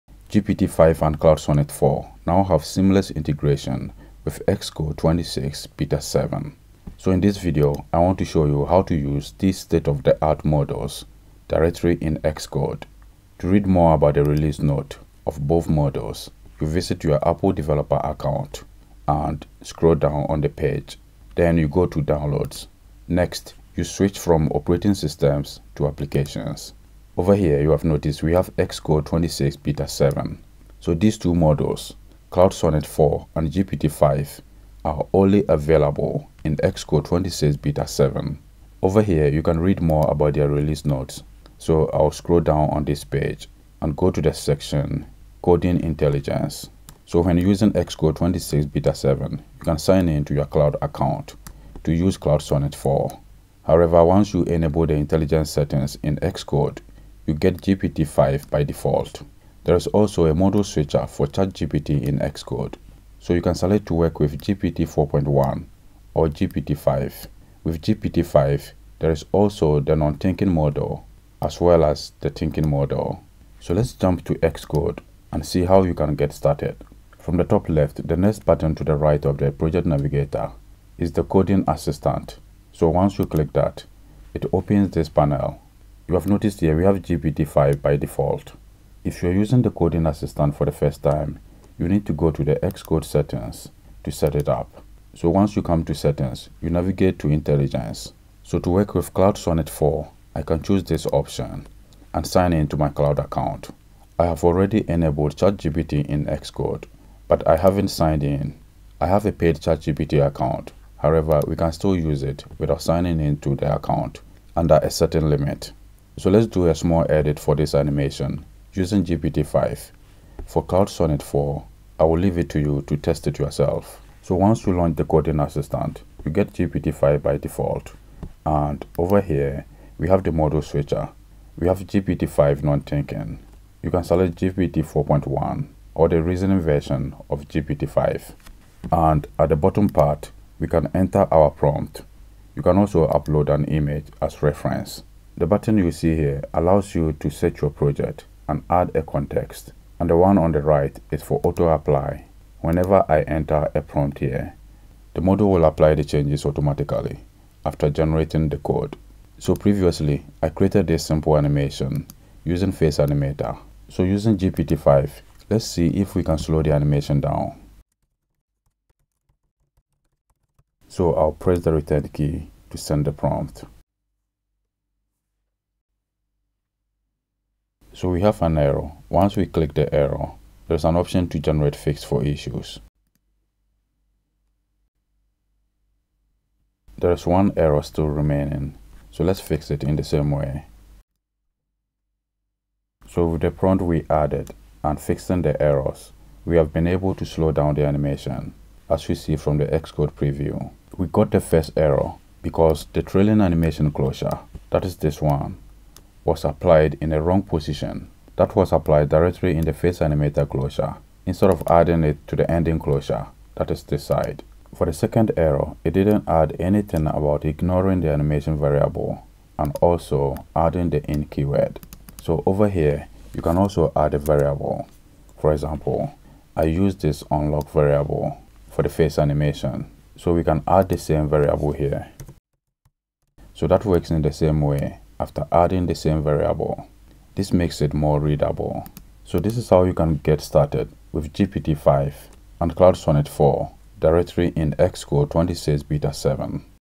Use GPT-5 & Claude Sonnet 4 in Xcode
In Xcode 26, you can use GPT-5 and Claude Sonnet 4 by connecting your model provider's account. Let's look at how to get started in this video.

Learn more about my SwiftUI and AI courses on Udemy.

GitHub Repos.